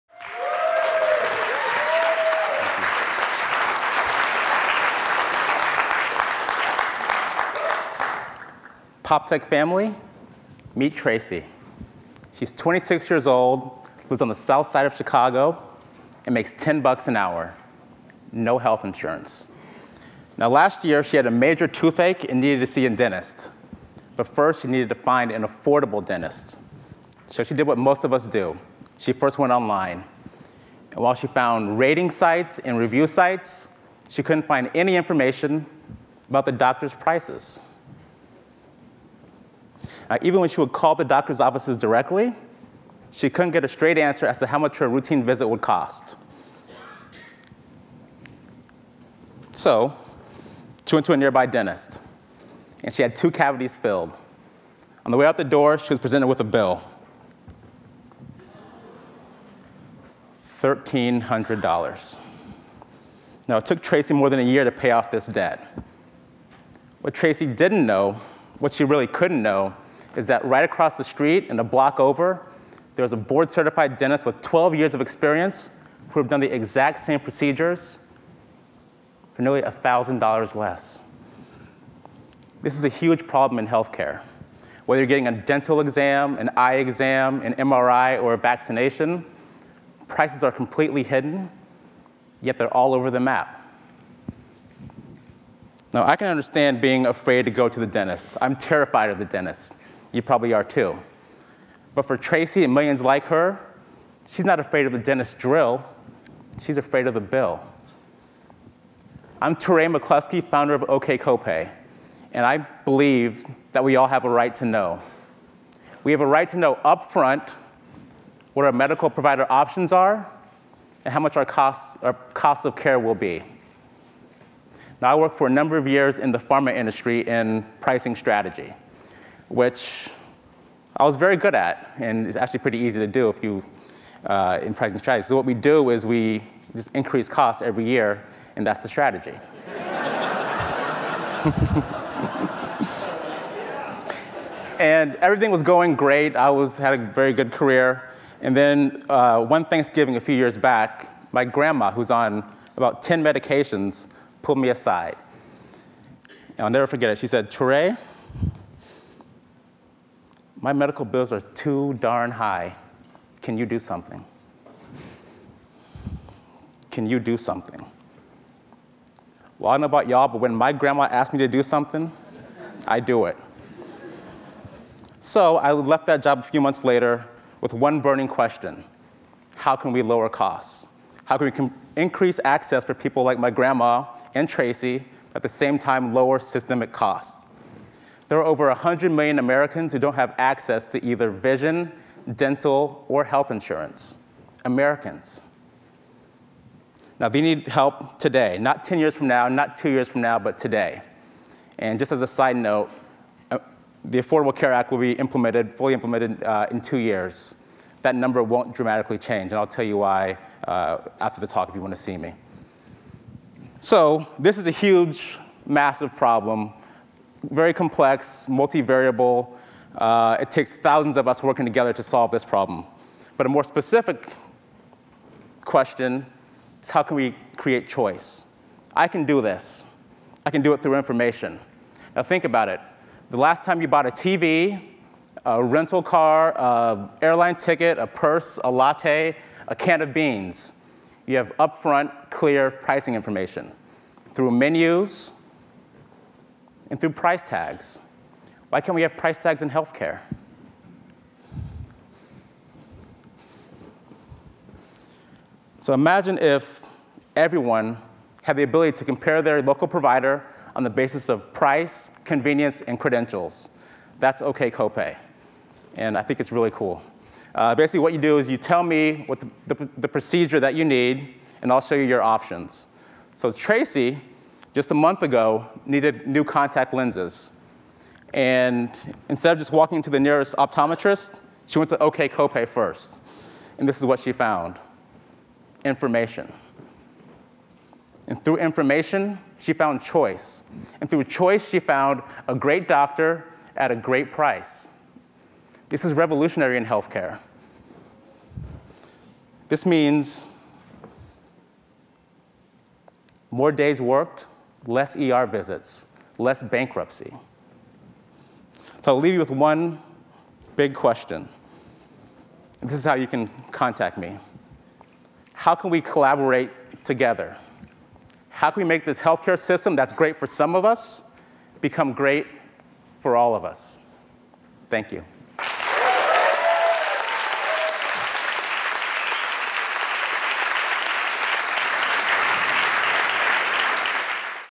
Touré McCluskey's healthcare shopping
Touré McCluskey is the founder of OkCopay, a unique search engine for medical procedures that helps Americans with inadequate insurance find affordable local health care. "Whether you are getting a dental exam, an eye exam, an MRI or a vaccination, prices are completely hidden, yet they are all over the map."